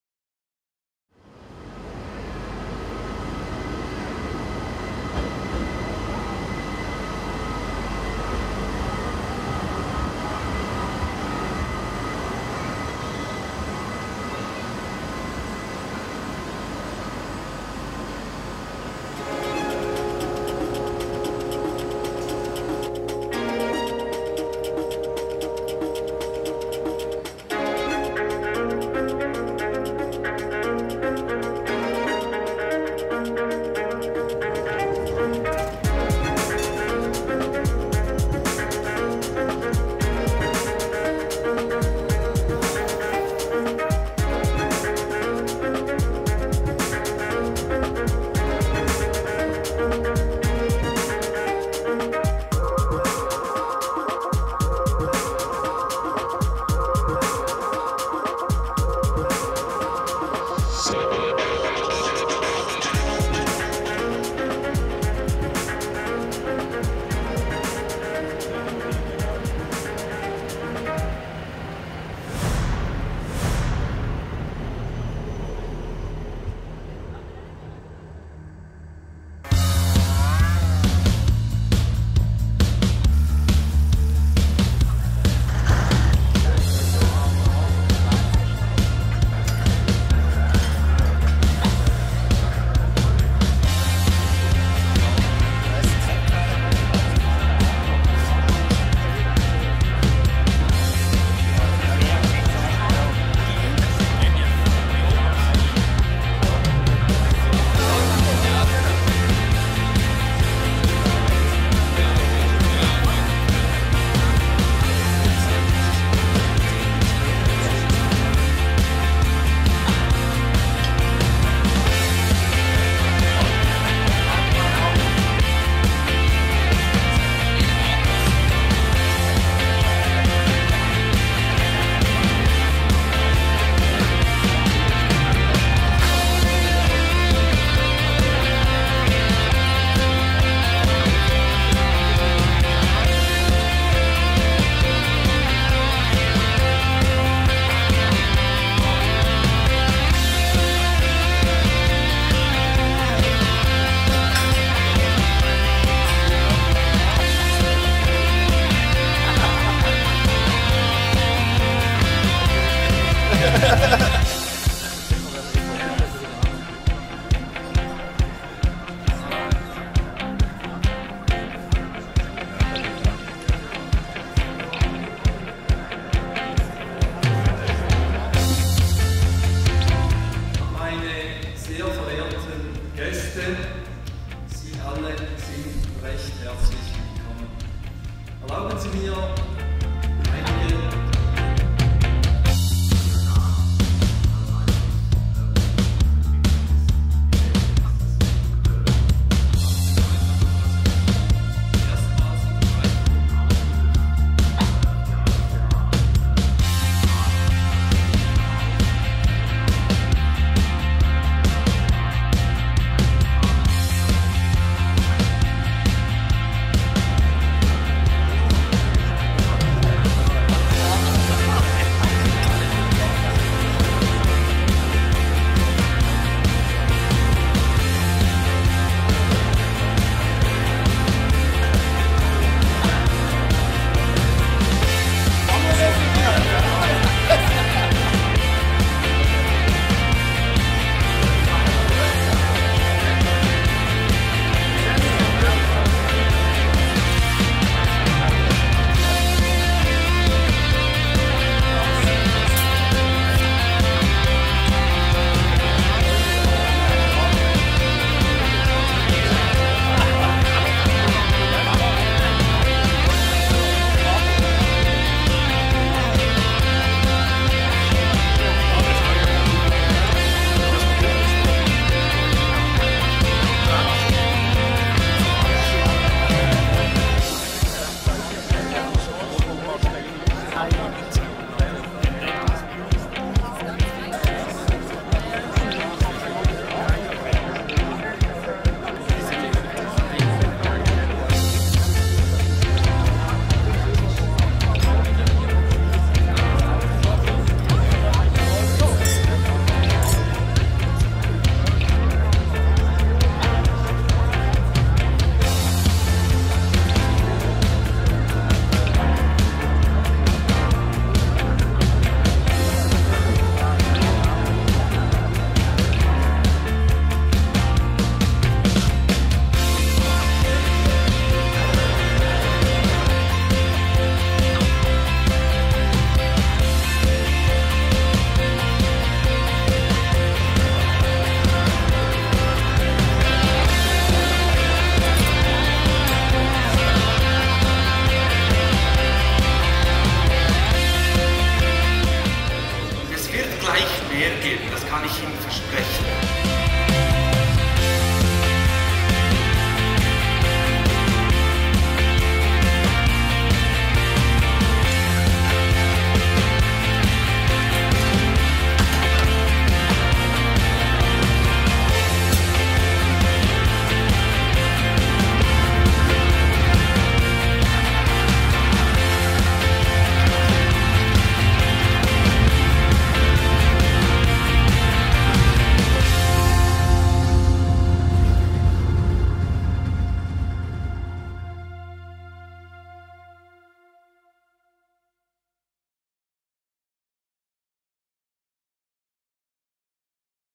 Generalversammlung VöV 2013 in Bern
Bern ist einer der wichtigsten öV-Verkehrsknotenpunkte des Landes. Von den internationalen TGV's und den Fernverkehrszügen der SBB im Bahnhof Bern über die regional verankerten Bahnunternehmen BLS AG und Regionalverkehr Bern--Solothurn (RBS) oder die Postautos bis hin zu den roten Trams und Bussen von BERNMOBIL vereinigt Bern alle Arten von öffentlichem Verkehr mit dem Bahnhof als Dreh- und Angelkreuz. Dieses Jahr war Bern der Austragungsort der VöV-Generalversammlung 2013.

Der Film präsentiert eine Zusammenfassung der VöV-Generalversammlung in Bern mit den wichtigsten Highlights hautnah. Entdecken Sie die Bundeshauptstadt aus dem Blickwinkel des öffentlichen Verkehrs.